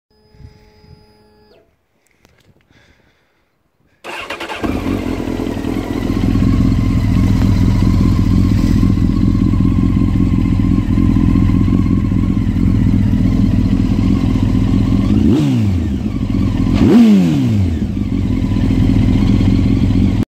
Honda CBR954RR 2002 with Stark dual muffler soundclip
Heres my CBR954RR 2002 with Stark dual mufflers. Enjoy